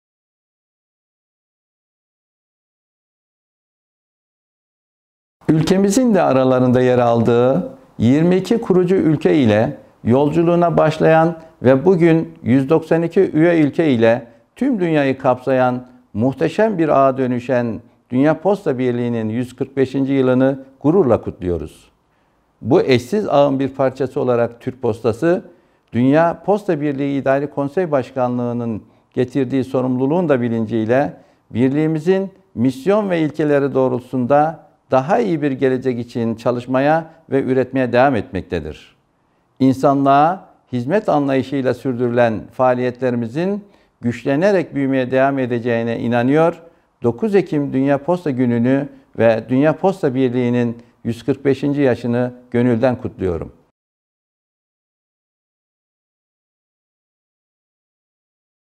World Post Day Message from the CA Chairman
Message from Kenan Bozgeyik, Chairman, UPU Council of Administration and Chairman of the Board and Director General, Turkish Post